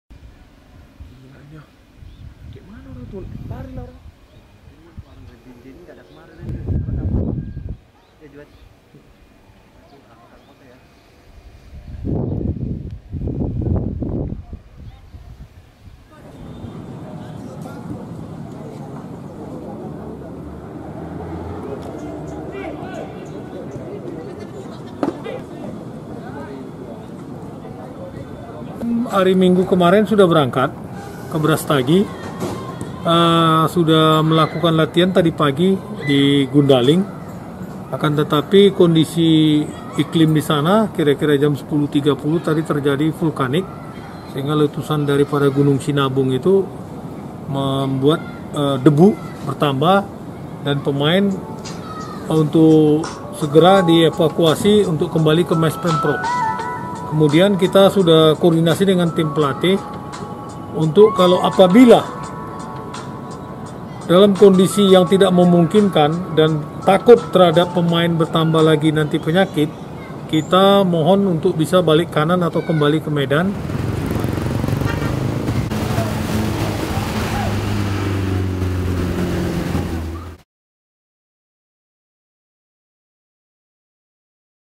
Menghindari Dampak Erupsi Sinabung, Latihan PSMS Medan di Berastagi Terancam Batal
KARO, KOMPAS.TV - Untuk meningkatkan daya tahan, stamina, dan VO2MAX para pemain, tim sepak bola PSMS Medan menggelar latihan di daerah dataran tinggi Berastagi, Kab Karo.

Namun latihan intensif yang direncanakan selama satu minggu tersebut, terancam batal karena erupsi Gunung Sinabung  yang terus terjadi.

Menurut rencana, manajemen PSMS Medan akan mengalihkan lokasi latihan.

Gunung Sinabung yang berada di Kabupaten Karo, Sumatera Utara, kembali erupsi pada Senin (10/8) pukul 10.00 WIB.

Pusat Vulkanologi dan Mitigasi Bencana Geologi (PVMBG) mencatat, erupsi kali ini menghasilkan kolom abu dengan ketinggian mencapai 5 kilometer.

Pada saat erupsi, angin mengarah ke timur dan tenggara gunung.

Dari pantauan petugas, usai erupsi masih terjadi gempa-gempa vulkanik di Gunung Api Sinabung.

Gunung Sinabung saat ini berada pada level 3 atau siaga.

Warga dihimbau untuk tidak beraktivitas di zona merah Gunung Sinabung. (*)